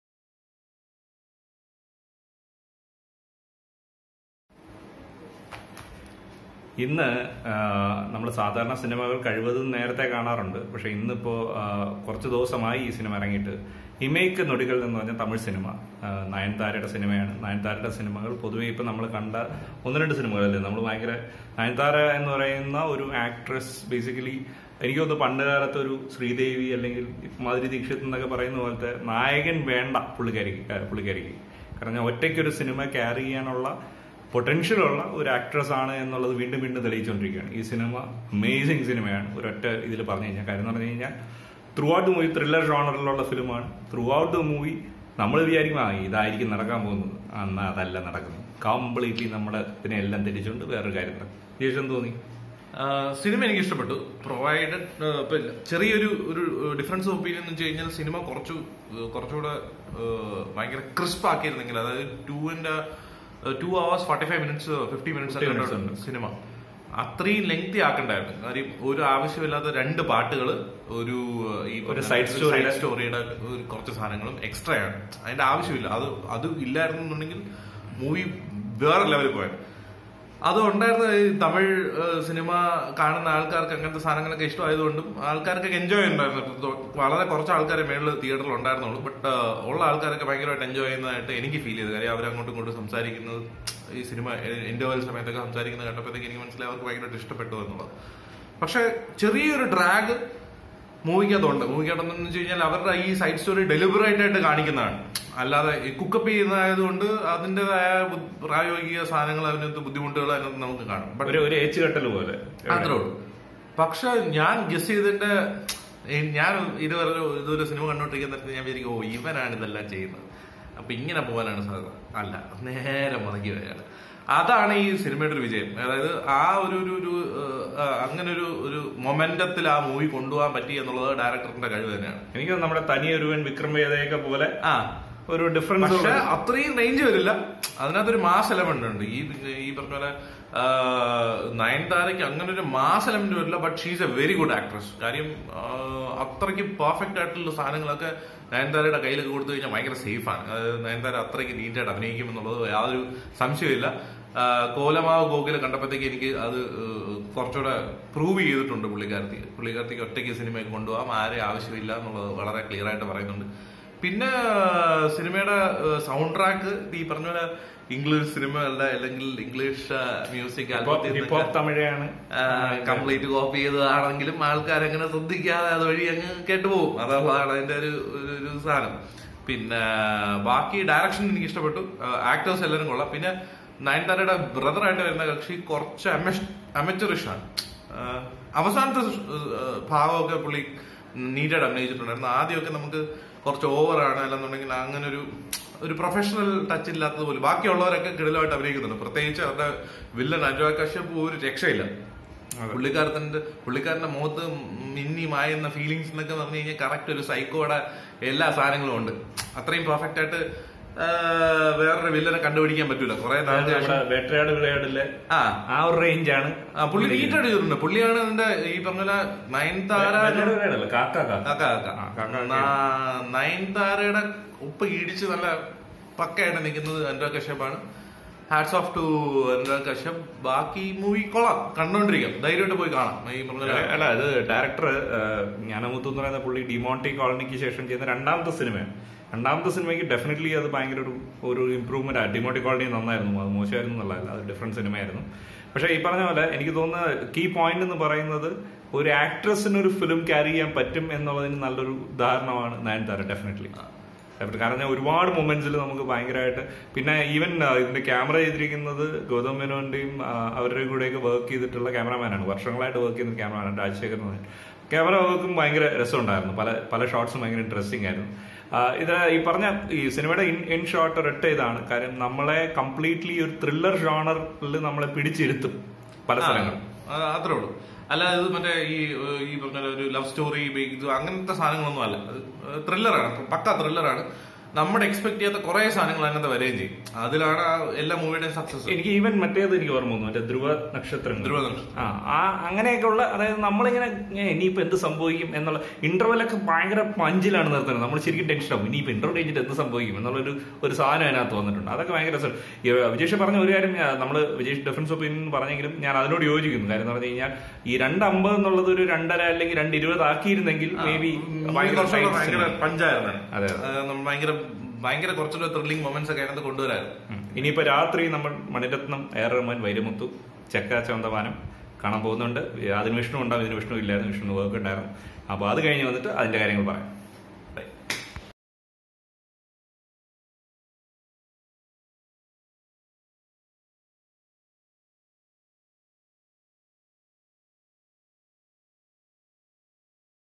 Imaikkaa Nodigal - A Clapboard Take / Review
Rudra is a serial killer with a unique way of killing his victims. Anjali is a CBI officer who investigates the case. Martin is the police officer who had done the initial investigation. What connects them?